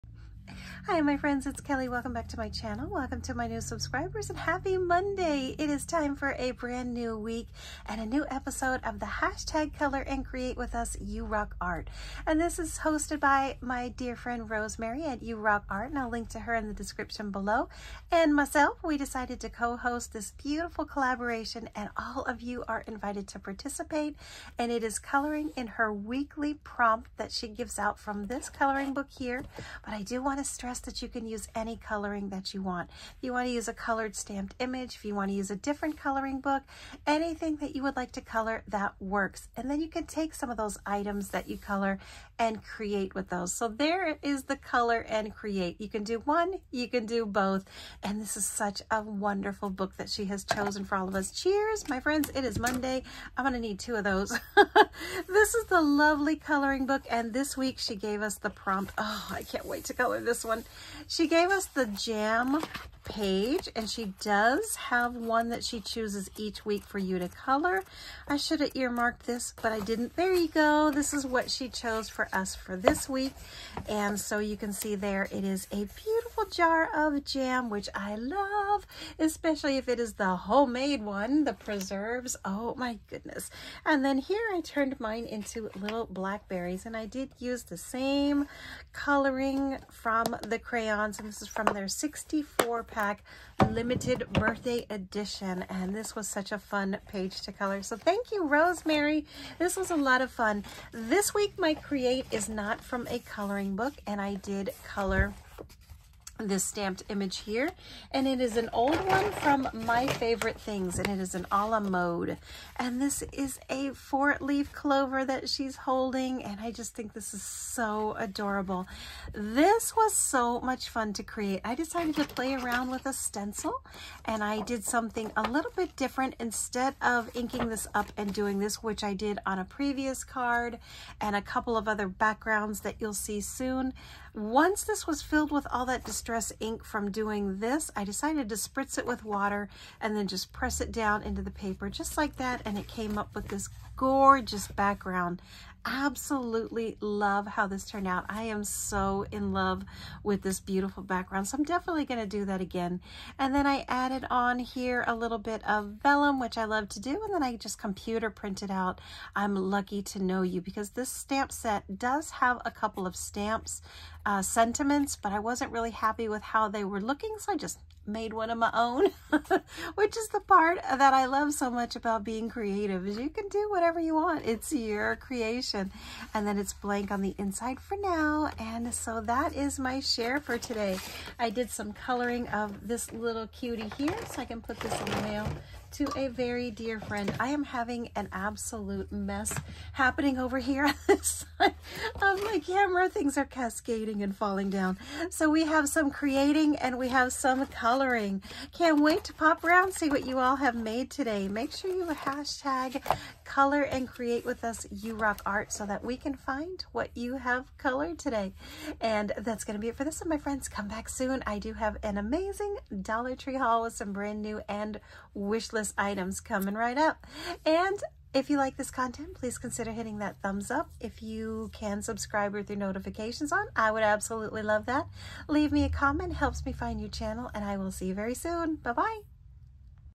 You're my JAM! colorandcreate ~ Week 2!
Hi friends Happy Monday and welcome to week 2 of colorandcreatewithusurockart hosted by my friend @rosemarysurockart2111 and cohosted by me @createwithkas
This week's prompt:  JAM!
 I also have a card share:  company mentioned @MFTKim and @marykayacolorfullifedesign2591

Use the hashtag colorandcreatewithusurockart and color and create!